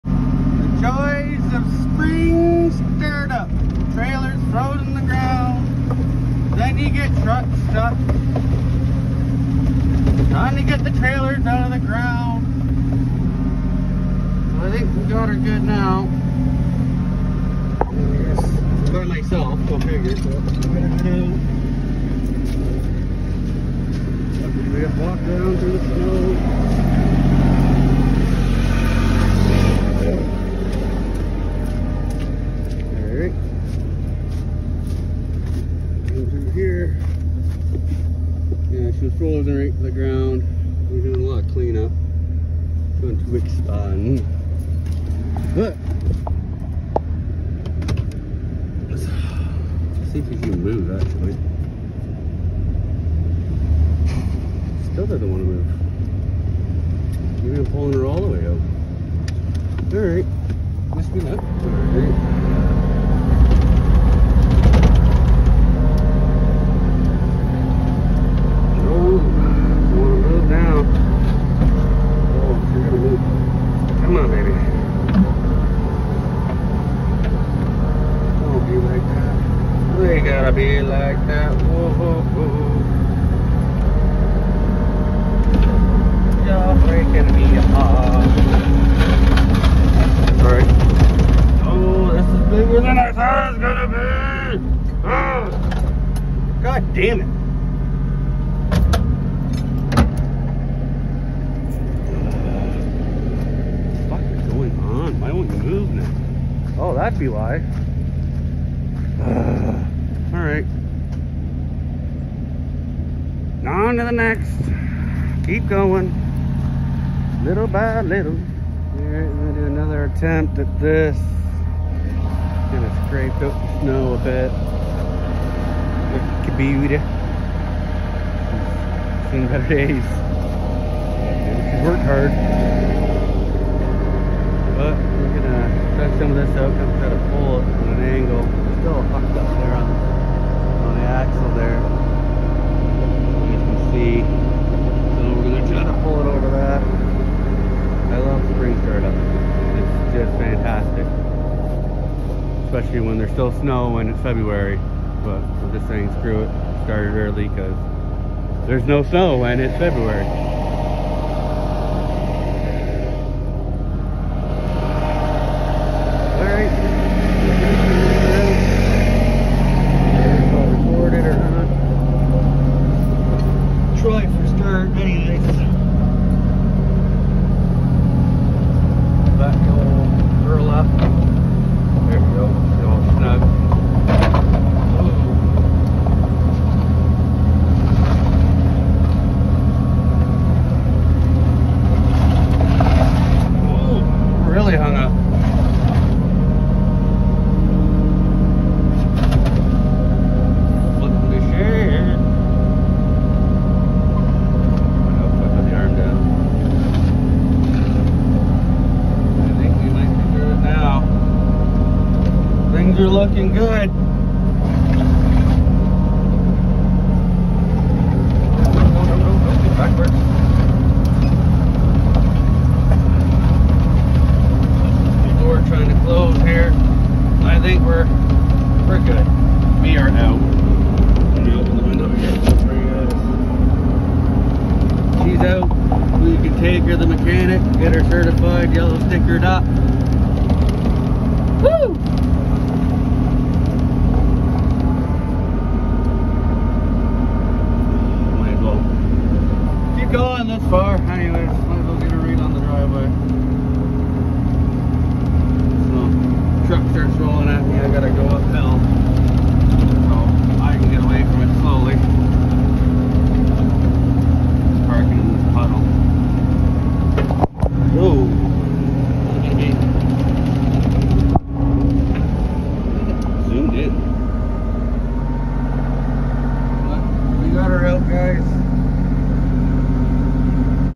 Trying to get the landscape trailer and get the truck stuck on ice and trailer hung up on snow bank
Spring time slowly coming it’s time to start pulling everything out to start preparing for this season’s landscaping work, so need to get the trailer out to get certified and of course I get stuck on the ice and trailer hung up on snow bank after I get it in frozen from the ice as well, dang don’t I love getting ready for spring time by my self